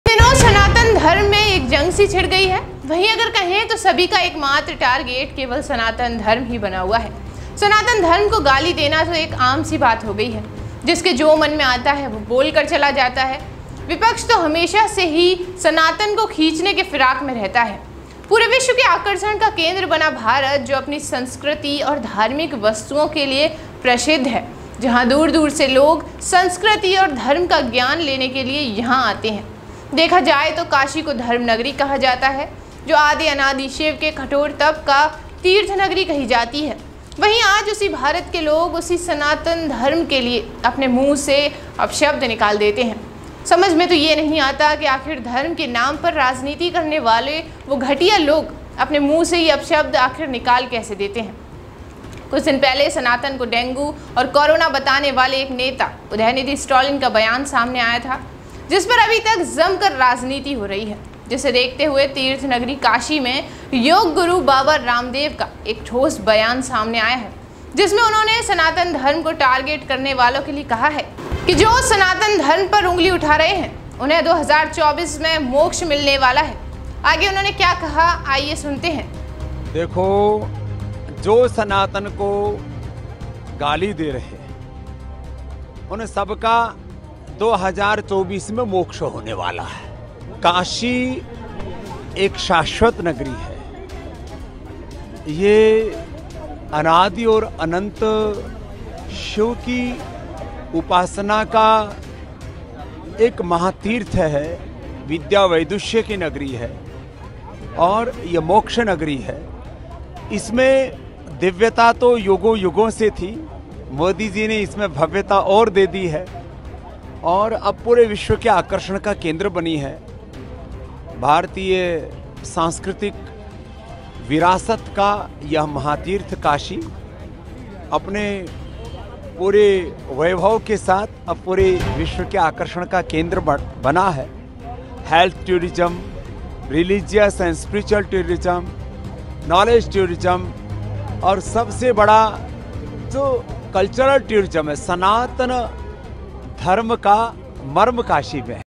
Varanasi :सनातन धर्म,2024 में उनको मिलेगा मोक्ष काशी में भड़के Baba Ramdev
breakingnews pmmodi Swami Ramdev ने 2024 के LokSabha चुनाव को लेकर दिया बड़ा बयान, मोक्ष मिलेगी...
Baba Ramdev on Sanatan Controversy: बाबा रामदेव (Baba Ramdev) ने कहा कि काशी (Kashi) अपने आप में दिव्य तो था ही पीएम नरेंद्र मोदी (PM Modi) ने काशी को भव्यता प्रदान की है। इसके साथ ही उन्होंने सनातन विवाद पर भी प्रतिक्रिया दी।

'सनातन धर्म को गाली देने वालों को 2024 में मोक्ष मिल जाएगा', काशी में भड़के Baba Ramdev